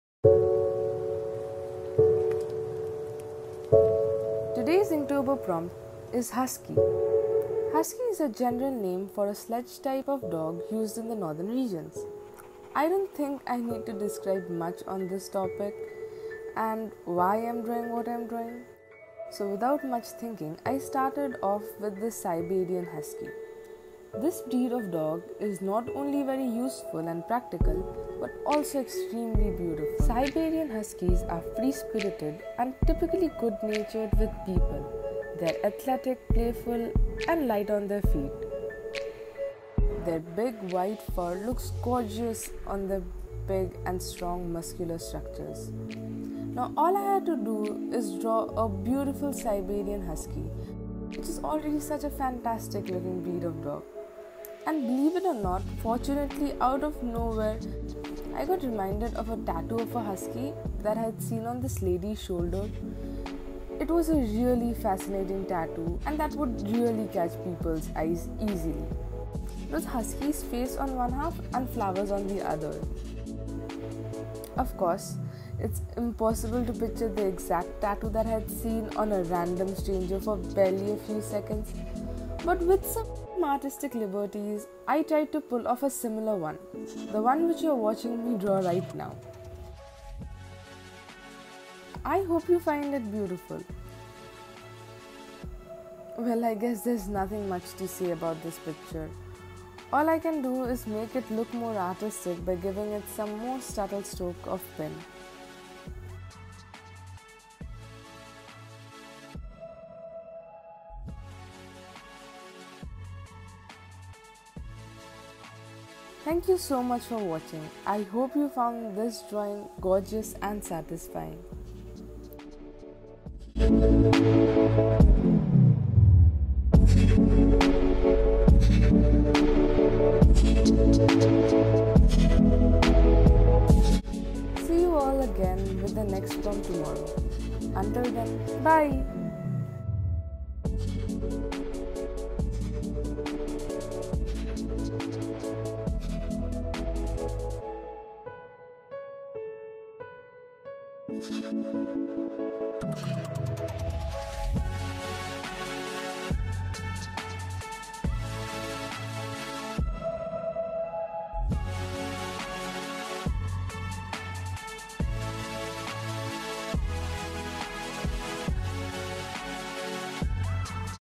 Inktober 2019 : Day 06 Husky || Sketchbook Corner
So I tried Inktober, for the first time! I am really not an illustrator, I'm more comfortable with drawing portraits and my illustration skills are Rough. I would really like to improve them though because it is something that I still enjoy and would love to do it more often. Inktober really stretched my illustration muscles for the first time in like years so it was fun for me! I'm struggling with completing the art, recording and then uploading everyday but its worth it!